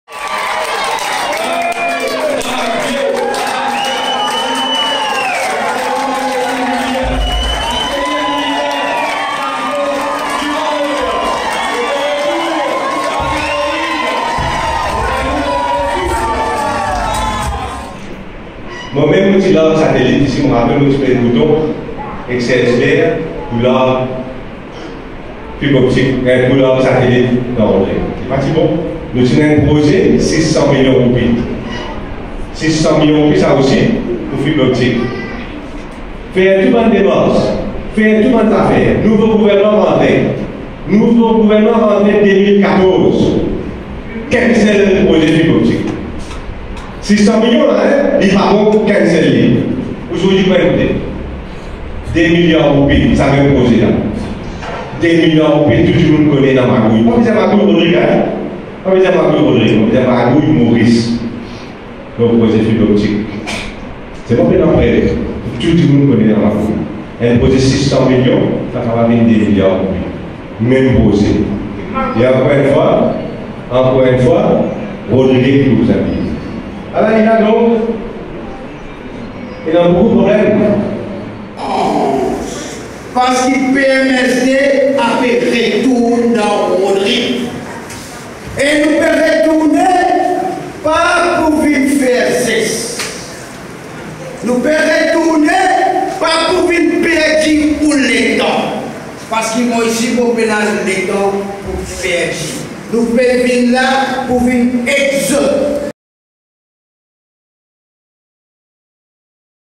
Quand Xavier Duval parle de son papa
Il a atterri à Rodrigues le mardi 22 janvier. Xavier Duval a tenu un congrès dans la soirée, où il n’a pas manqué de rappeler que c’est grâce à son père cette île a connu des développements à la fin des années 1960. Plus d’un millier de personnes avaient fait le déplacement pour l’écouter.